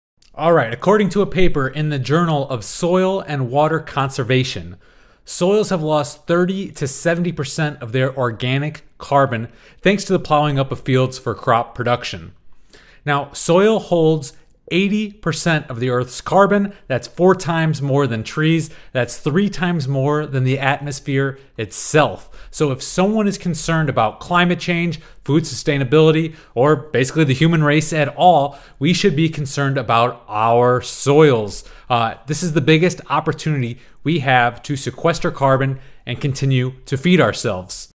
Soil Degradation - The Biggest Threat to Food Sustainability
If the soils disappear, so do we.

We need more properly managed ruminants to regenerate soils, sequester carbon, provide protein, and give us a chance at food sustainability.